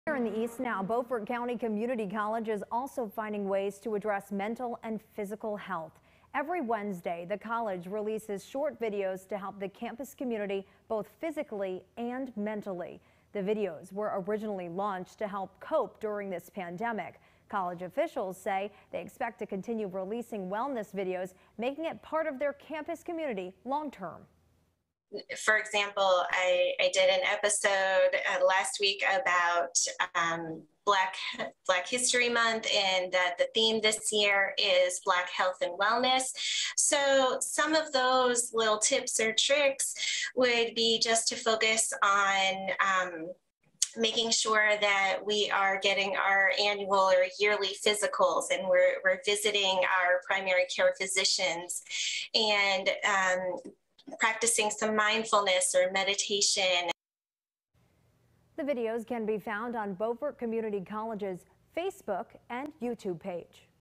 Beaufort County Community College brings attention to mental and physical health
Beaufort County Community College is finding ways to address mental and physical health.